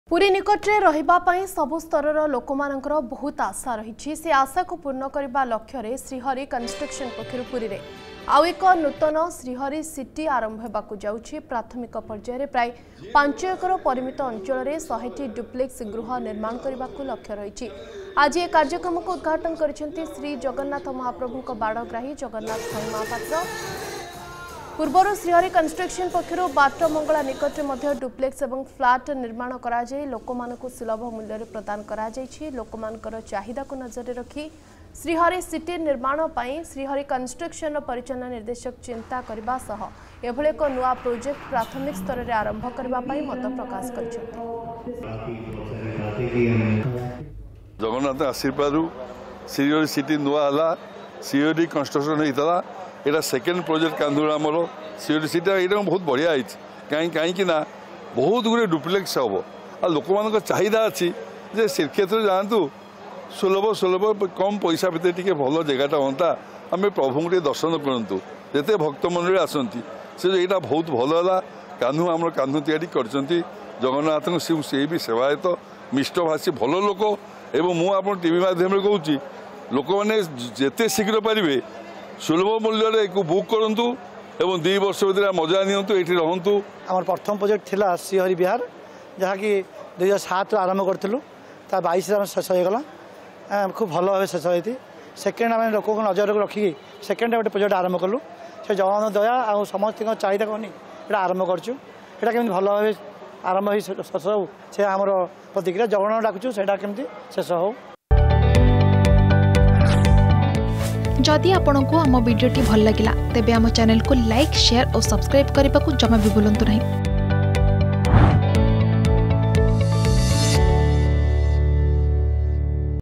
ପୁରୀରେ ନୂତନ ଶ୍ରୀହରି ସିଟି || Shreehari City || Puri
ଶ୍ରୀହରି କନଷ୍ଟ୍ରକ୍ସନ ପକ୍ଷରୁ ପୁରୀରେ ଆଉ ଏକ ନୂତନ ଶ୍ରୀହରି ସିଟି ଆରମ୍ଭ ।  ପ୍ରାୟ ୫ ଏକର ପରିମିତ ଅଞ୍ଚଳରେ 100ଟି ଡୁପ୍ଲେକ୍ସ ଗୃହ ନିର୍ମାଣ କରାଯିବାକୁ ଲକ୍ଷ୍ୟ ।